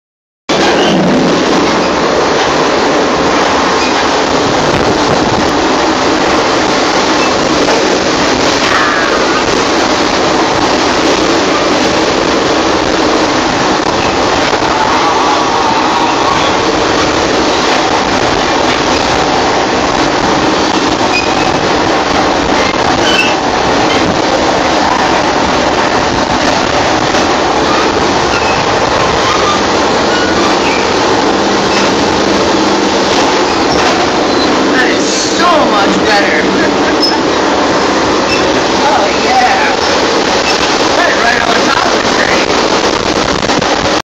head out train window, 2016
on way to Carolina Renaissance Festival, back when I was FlyingAxblade